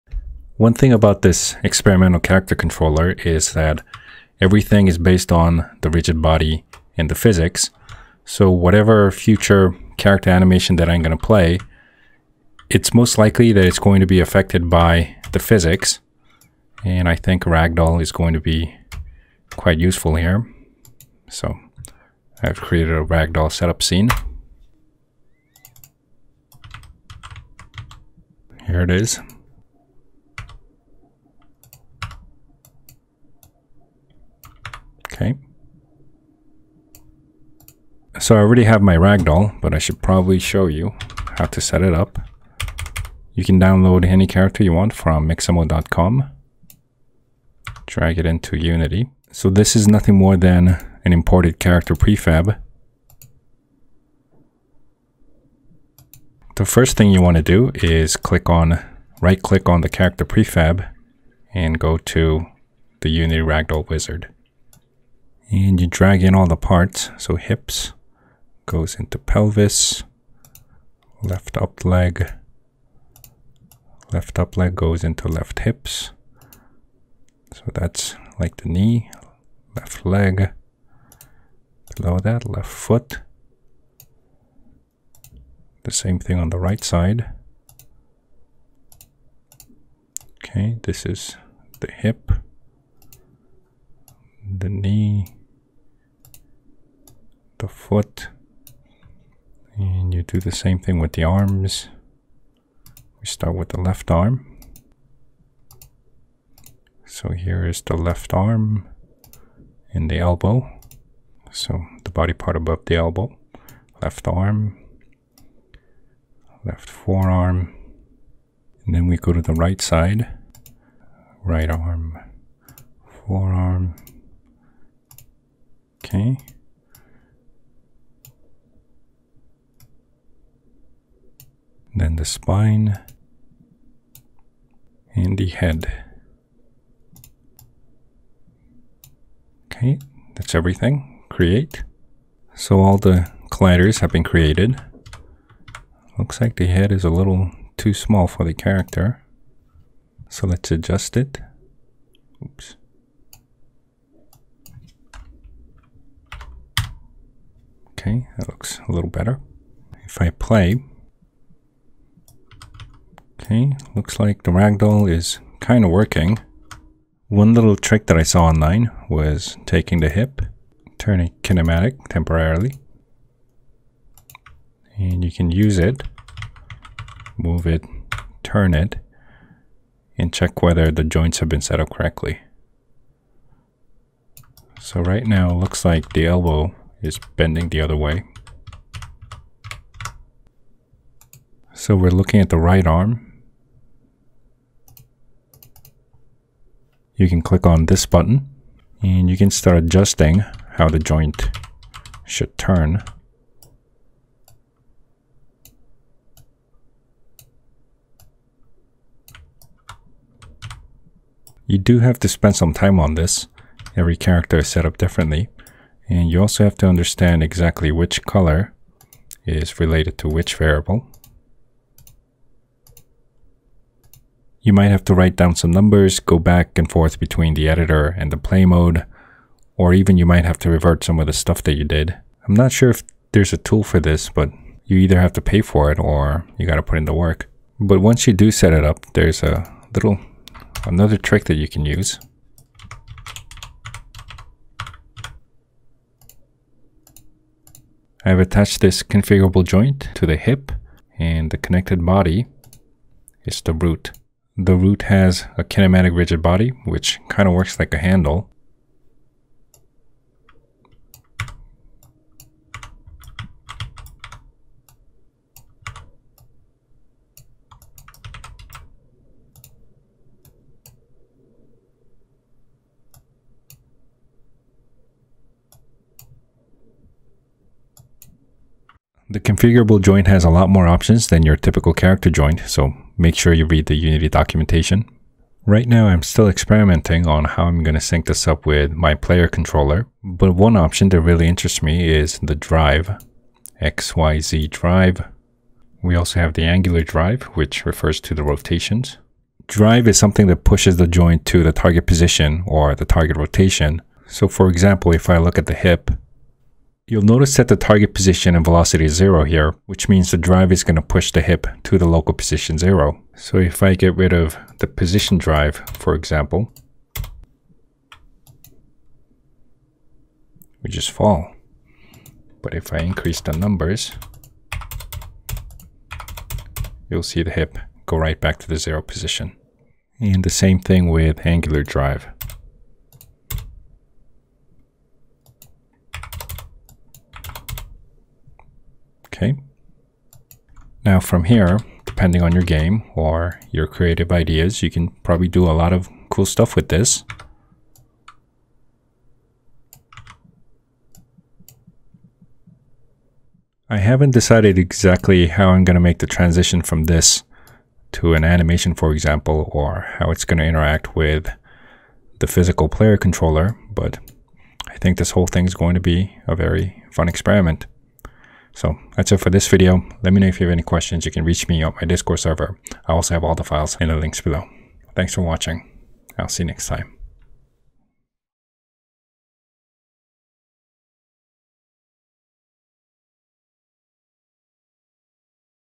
#7 Setting Up for Ragdoll Physics - Unity Tutorial
This is just one example, so be as creative as you want with the configurable joints. Once you get the hang of using them you'll probably start building cool stuff like active ragdolls and more.